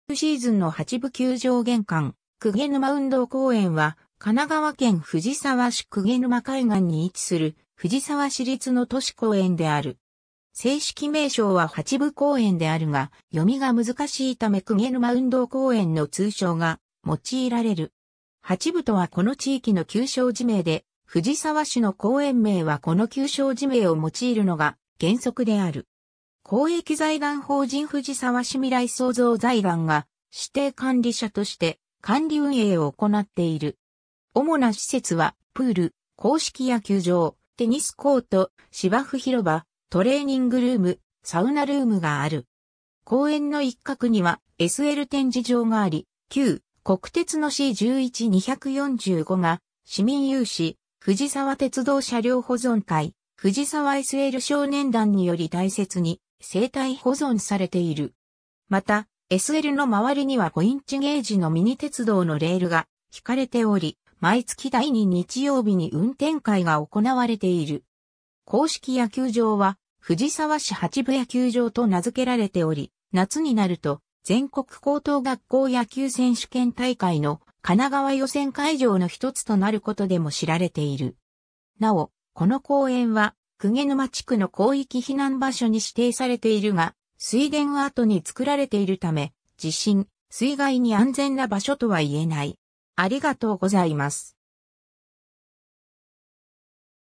鵠沼運動公園
高校野球シーズンの八部球場玄関 鵠沼運動公園（くげぬまうんどうこうえん）は、神奈川県藤沢市鵠沼海岸に位置する藤沢市立の都市公園（運動公園）である。
正式名称は八部公園（はっぺこうえん）であるが、読みが難しいため鵠沼運動公園の通称が用いられる。
八部とはこの地域の旧小字名で、 藤沢市の公園名はこの旧小字名を用いるのが原則である。
 公益財団法人藤沢市みらい創造財団が、指定管理者として管理運営を行っている。
 主な施設は、プール、硬式野球場、テニスコート、芝生広場、トレーニングルーム、サウナルームがある。
 公園の一角にはSL展示場があり、旧国鉄のC11 245（しおかぜ号）が市民有志、藤沢鉄道車両保存会・藤沢SL少年団により大切に静態保存されている。
また、SLの周りには5インチゲージのミニ鉄道のレールが引かれており、毎月第二日曜日に運転会が行われている。
 公式野球場は藤沢市八部野球場と名づけられており、夏になると全国高等学校野球選手権大会の神奈川予選会場の一つとなることでも知られている。
 なお、この公園は鵠沼地区の広域避難場所に指定されているが、 水田跡に造られているため、地震、水害に安全な場所とはいえない。